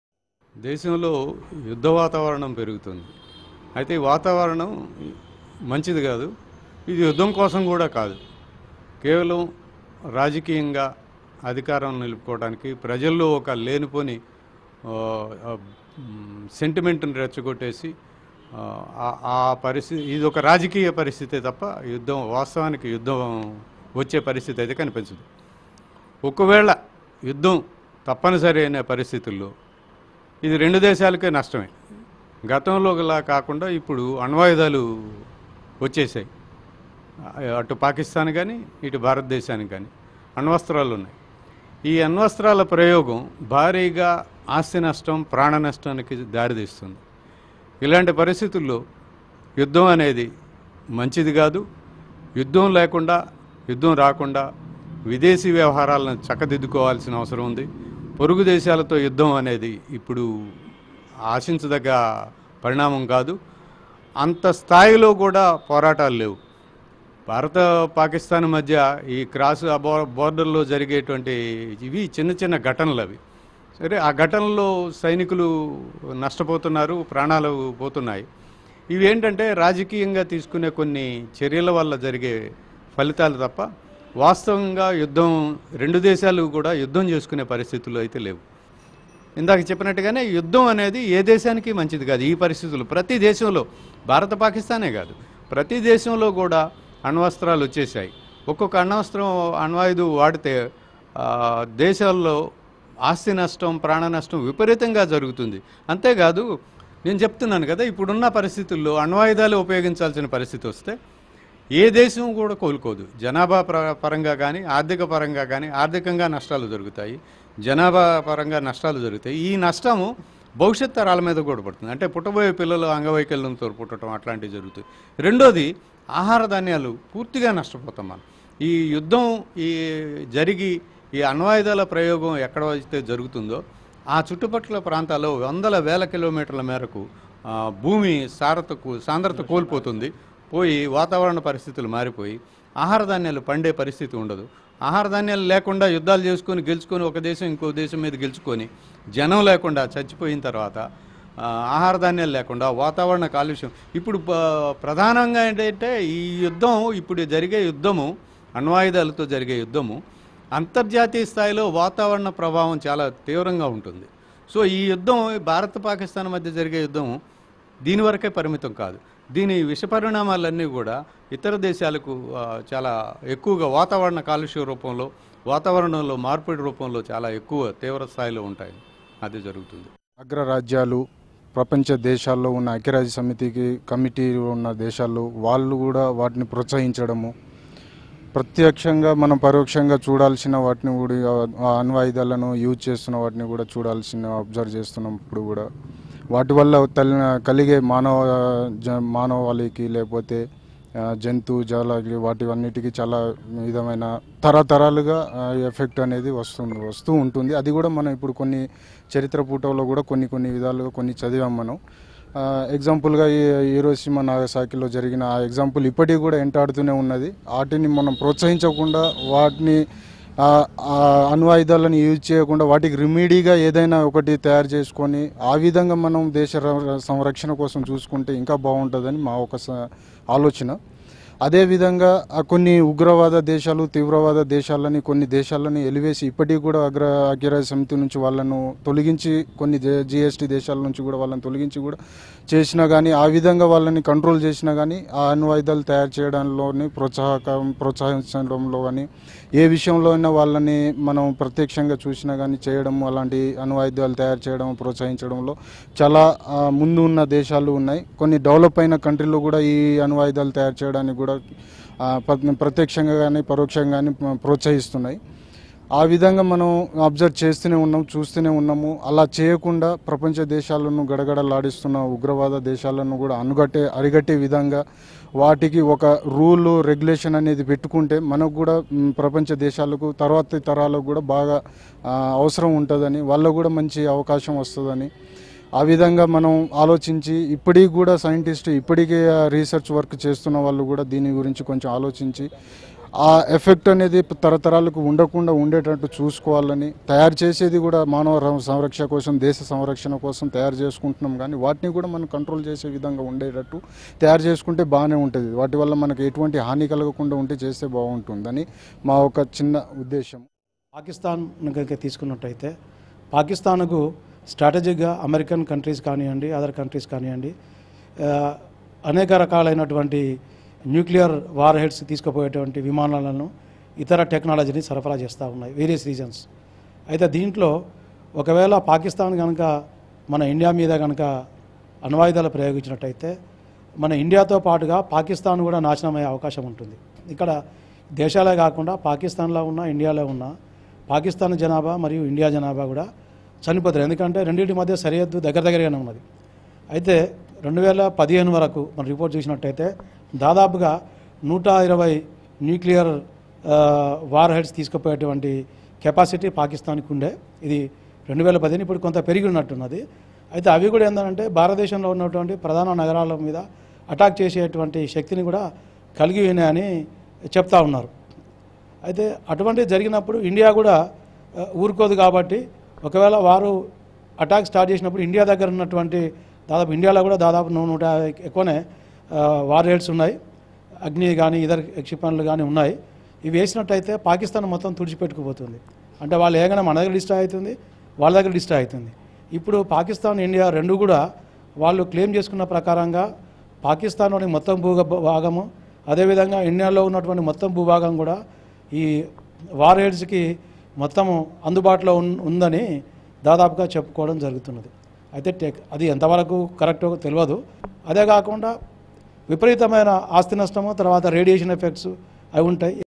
Indian People Opinion On What Will Happen if India-Pakistan Break into a Nuclear War | Bhaarat Today
Indian People Opinion On What Will Happen if India-Pakistan Break into a Nuclear War.